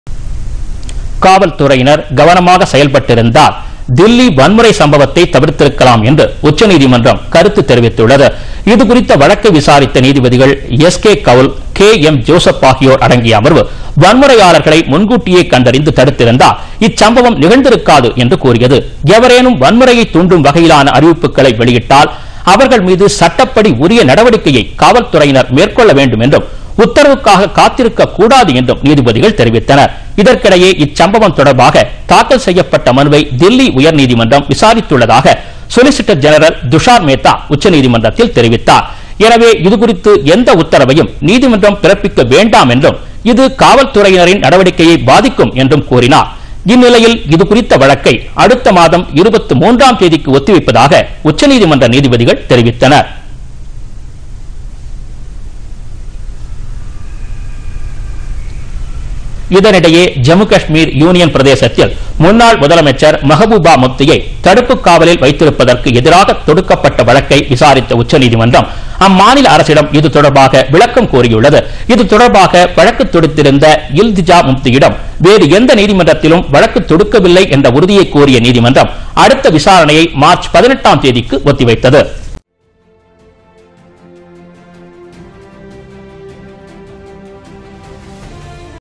காவல்துறையினருக்கு அறிவுரை
காவல்துறையினர் கவனமாக செயல்பட்டிருந்தால் தில்லி வன்முறை சம்பவத்தை தவிர்த்திருக்கலாம் - உச்சநீதிமன்றம் கருத்து.